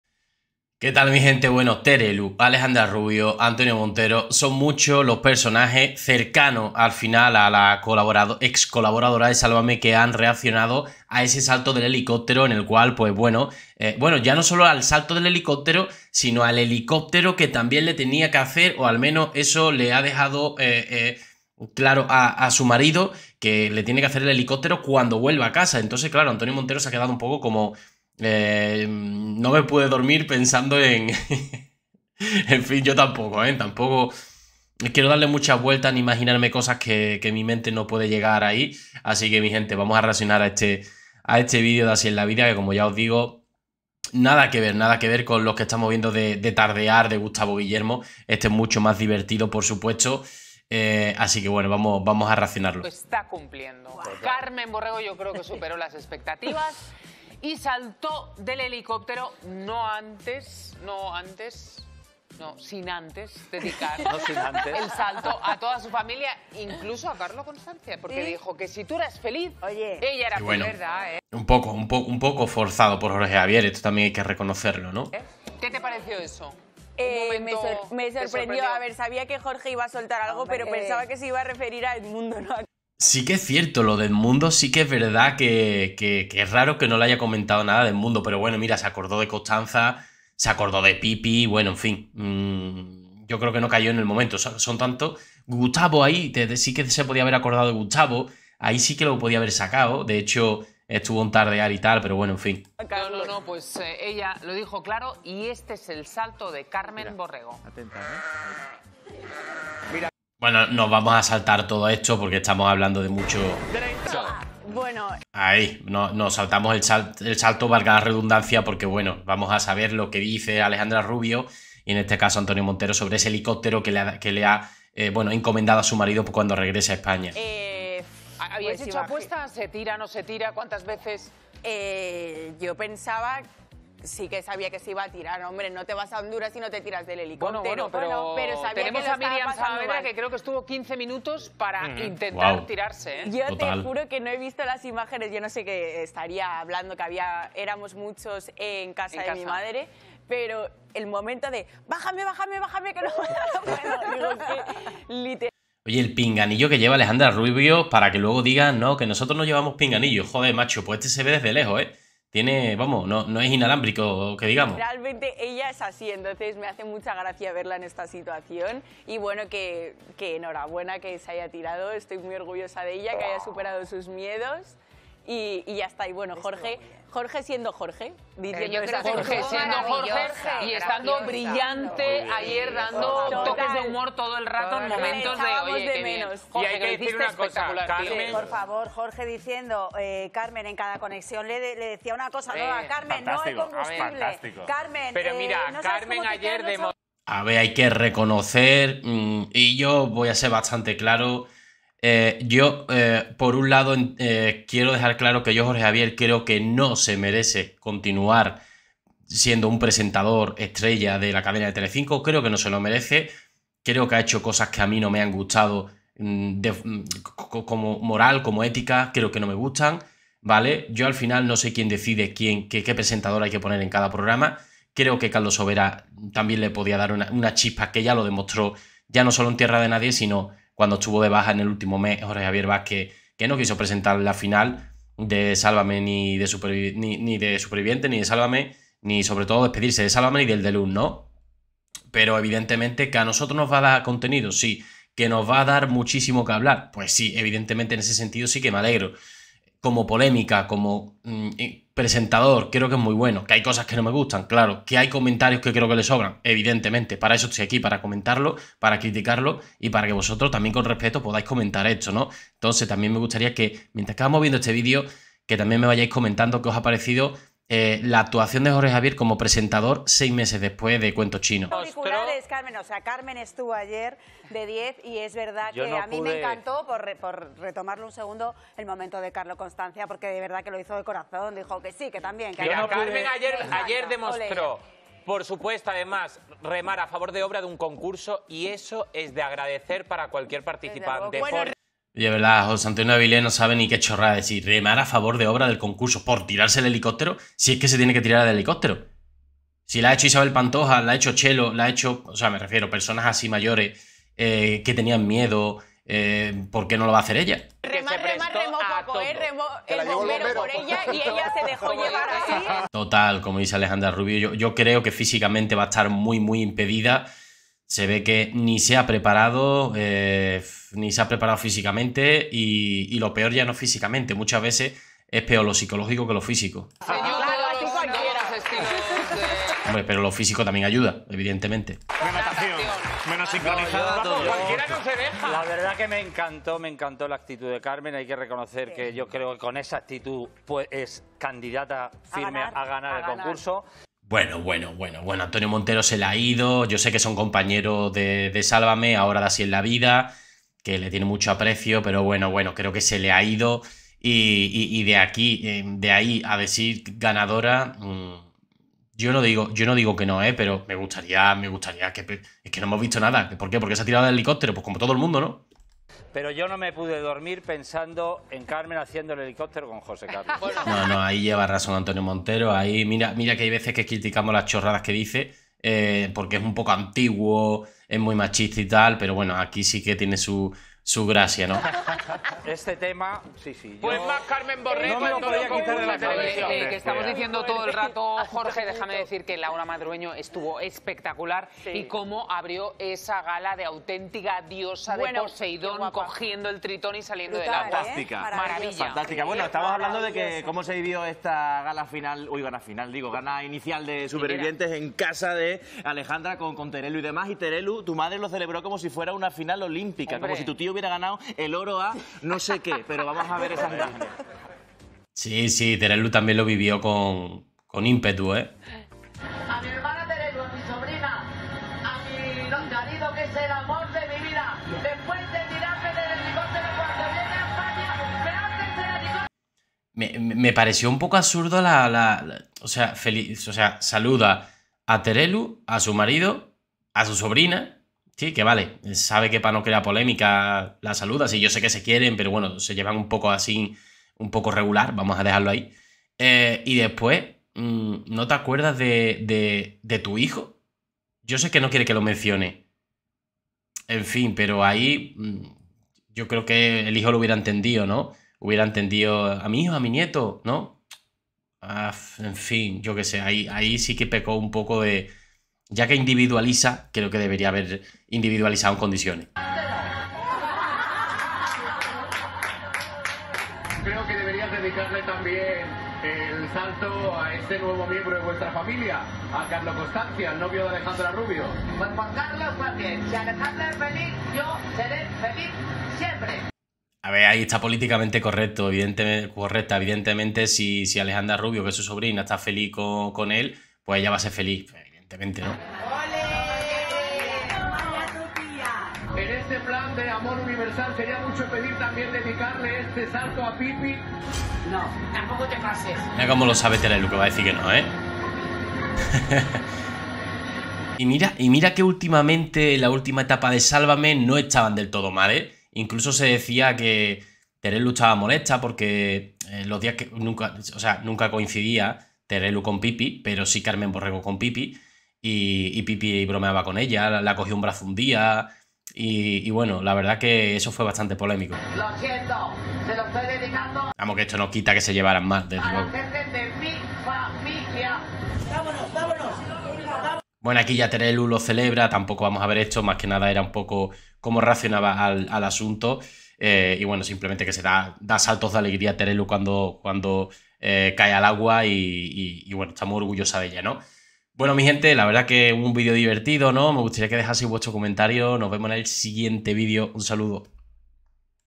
¡FLIPANTE! ALEJANDRA RUBIO LE SOPLAN LO QUE TIENE QUE DECIR SOBRE CARMEN BORREGO POR SUPERVIVIENTES
GRUPO WHATSAPP AVISOS DIRECTOS Y VÍDEOS👇
X @garcia_youtube
SI QUIERES SER INFLUENCER